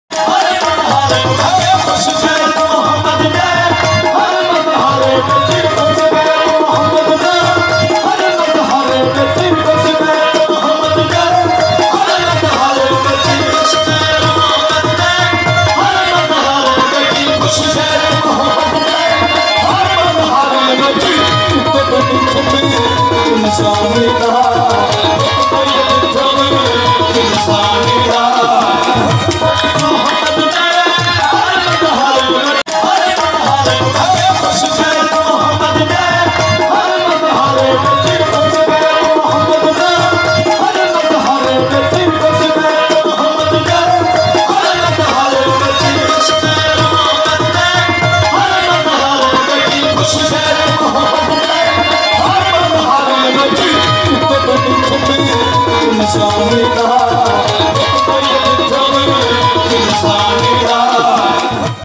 اختتامی محفل عرس حضرت شیخ خواجہ در محمد احمدانی
ملک بھر سے عقیدت مندوں کی شرکت